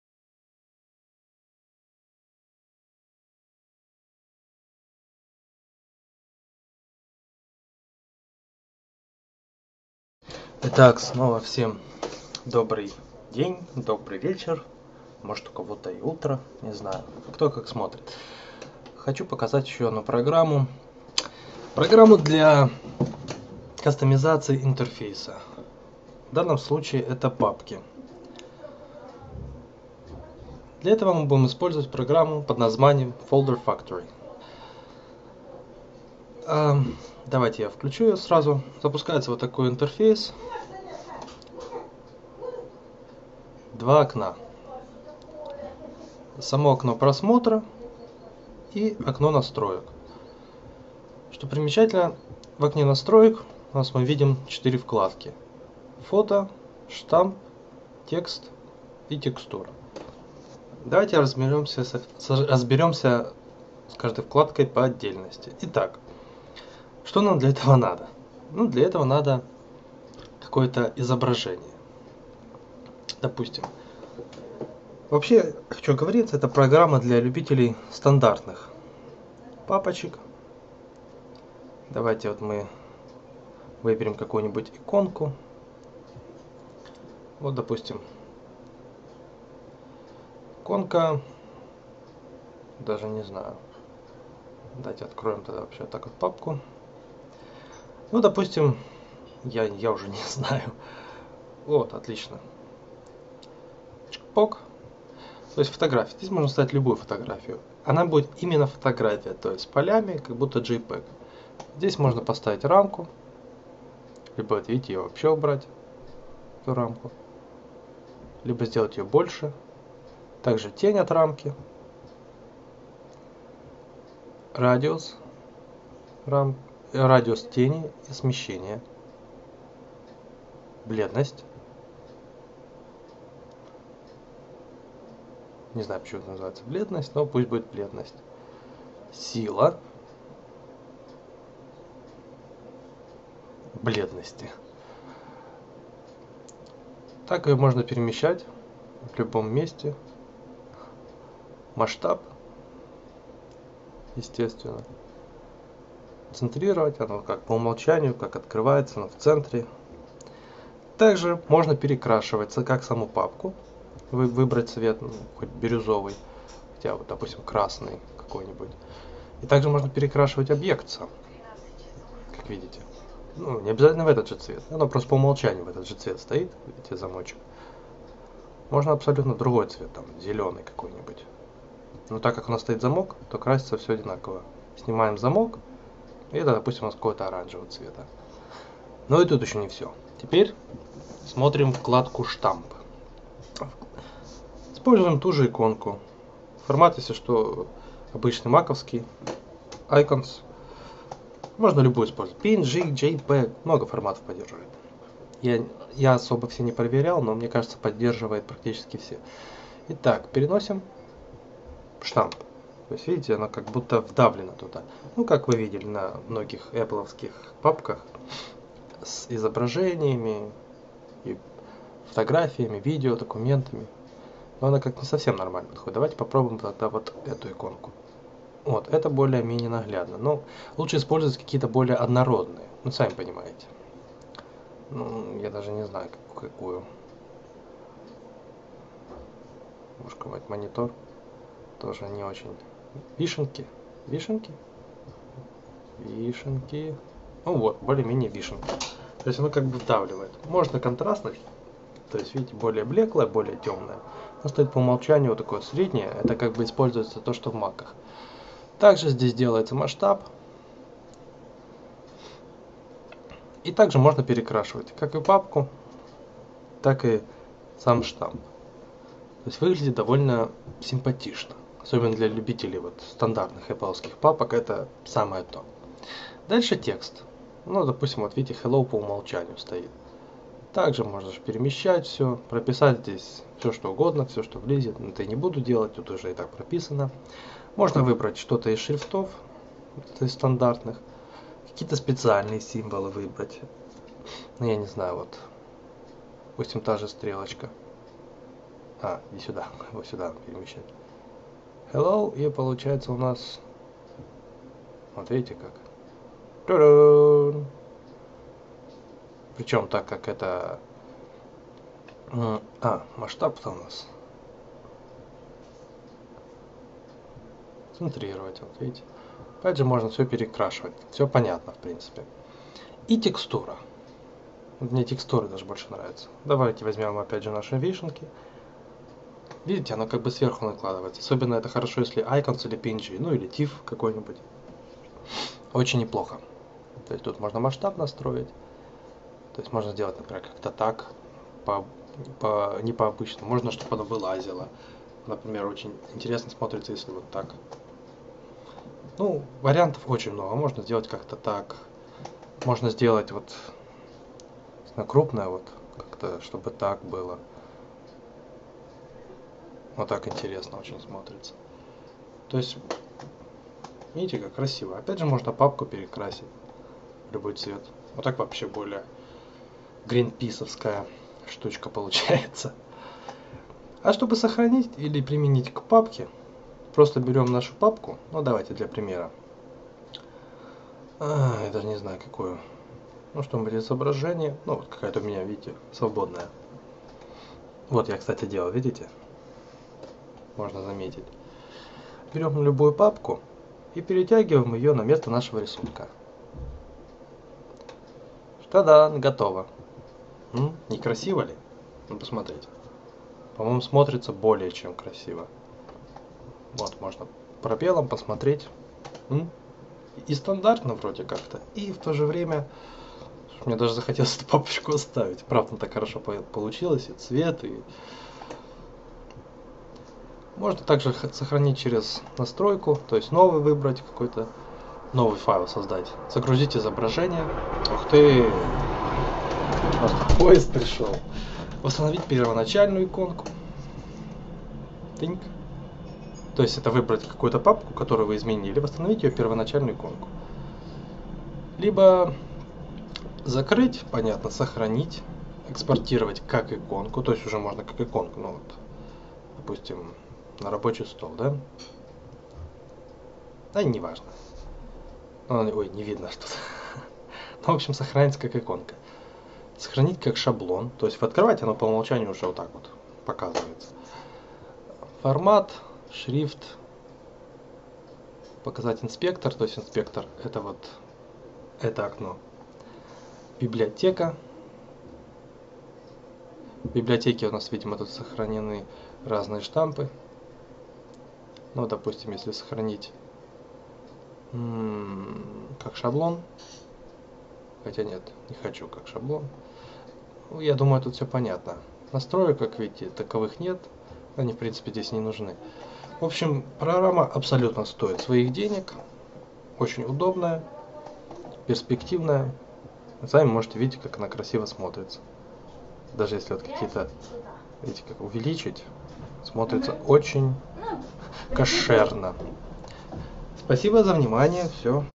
Обзор программы Folders Factory на OS X
Программа для кастомизации папок.
Цена: $2.99